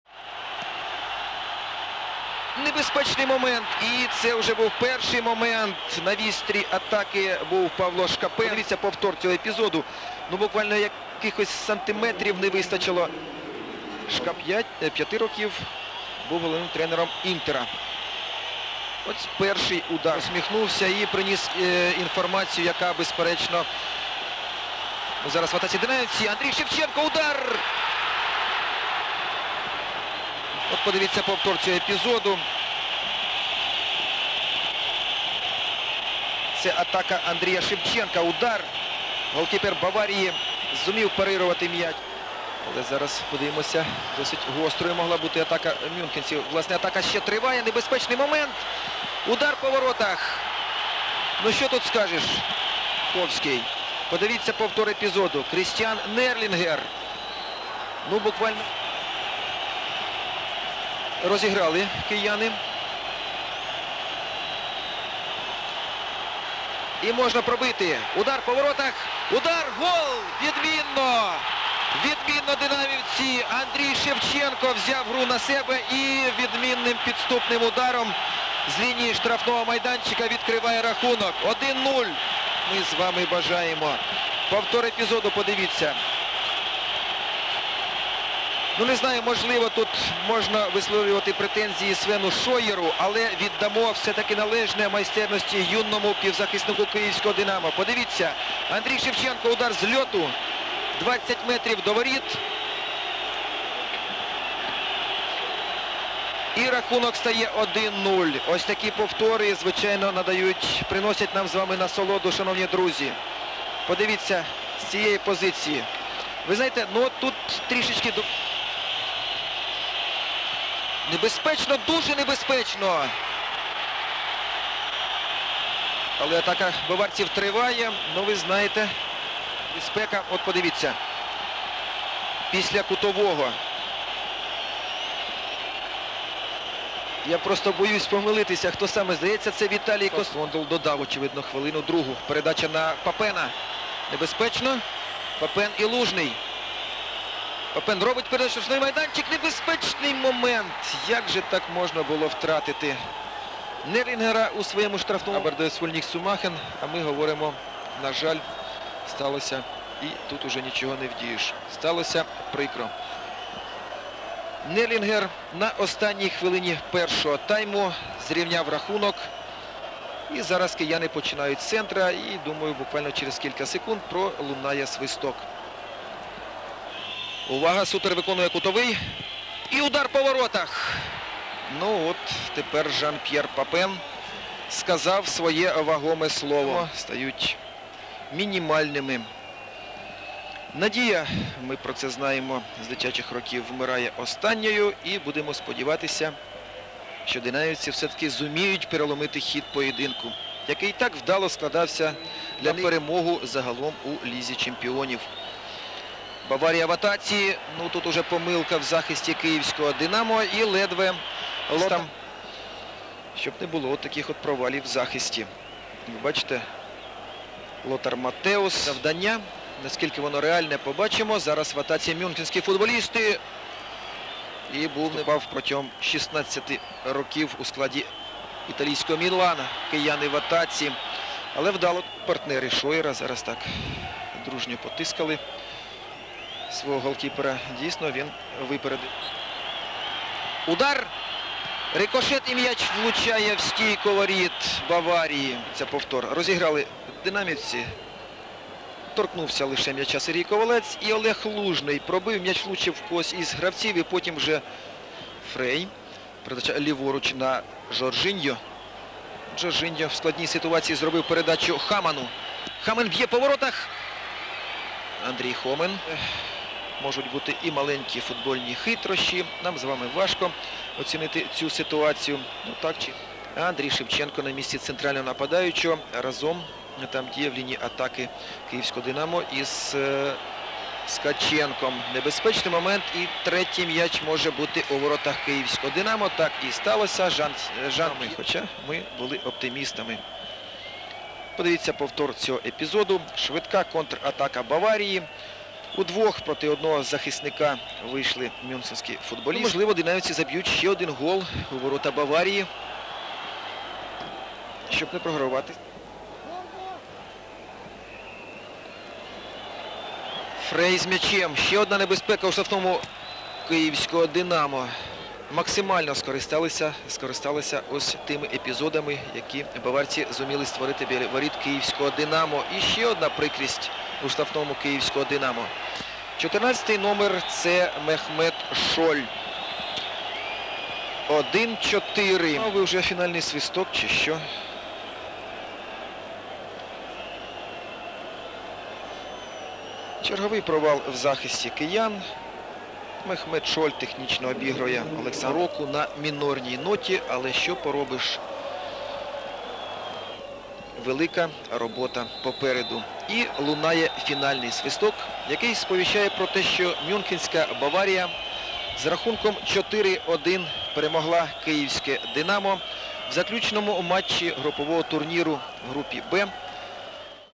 Динамо Киев - Бавария 1:4. ЛЧ-1994/95 (обзор матча)
07.12.94. Киев. Республиканский стадион. 52 000 зрителей. +5°C.

"Динамо": Шовковский Александр, Лужный Олег, Леженцев Сергей, Хомин Андрей, Михайленко Дмитрий, Ковалец Сергей, Косовский Виталий (Призетко Александр, 68), Скаченко Сергей, Шевченко Андрей, Леоненко Виктор (Мизин Сергей, 62), Шкапенко Павел.
Тренер: Йожеф Сабо.

"Бавария": Шойер, Баббель, Циге, Кройцер, Хелмер, Нерлингер, Жоржиньо, Хаманн, Папен, Маттеус (Фрей, 72), Суттер (Шолль, 78).
Тренер: Джовани Трапаттони.

Голы: 1:0 - Шевченко Андрей (38), 1:1 - Нерлингер (45), 1:2 - Папен (57), 1:3 - Папен (82), 1:4 - Шолль (87).

Арбитры: В. Крондл - О. Драстик и Й. Звонич (все - Чехия).

Предупреждены: нет - Суттер (31), Хаманн (42), Папен (52).

Й.Сабо: "Что можно сказать об игре, в которой команда вчистую проигрывает - 1:4 и показывает совершенно безвольную игру! После домашнего матча со "Спартаком", выигранного на чистом фарте, мои игроки распустились, начисто забыв об игровой дисциплине. У ребят начался тривиальный "звездняк". А он, как известно, к хорошему не приводит. Коллектив стал разваливаться, и главным его разрушителем стал Леоненко. Даже слепому должно быть видно, что Витя играет с шестью килограммами лишнего веса. Но, несмотря ни на что, я решился поставить его в стартовый состав. И убедился, что Леоненко потерял все свои былые качества - скорость, резкость и по уровню своей готовности способен подойти в лучшем случае нашей команде "Динамо-3". Толковой средней линии в "Динамо" нет, и с этим ничего не поделаешь. Разрушать умеют многие, а вот созидать - единицы. У Шкапенко сегодня было сто процентов технического брака. Не менее получаса я объяснял перед матчем Шкапенко и Михайленко, как они должны действовать против выдвинутых вперёд полузащитников "Байерна", однако установка не была выполнена и на десять процентов. В целом я доволен только игрой Шовковского и Шевченко".